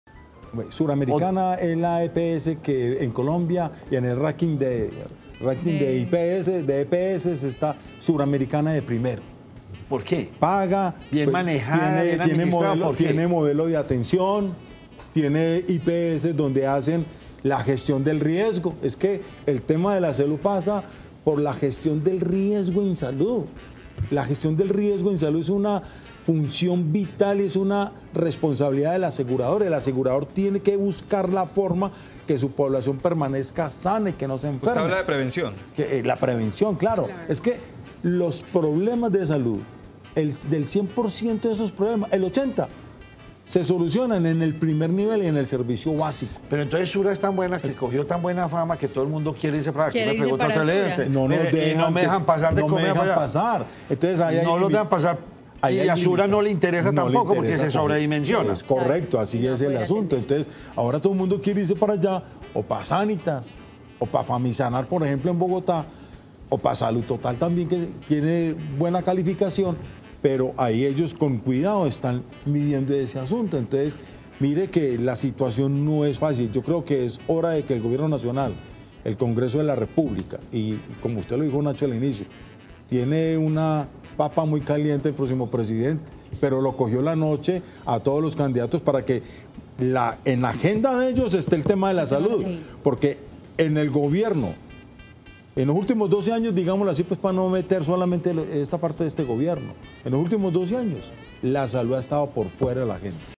La salud ha estado por fuera de la agenda del Gobierno en los últimos dos años
Tema tratado en el programa del miércoles 27 de septiembre de 2017.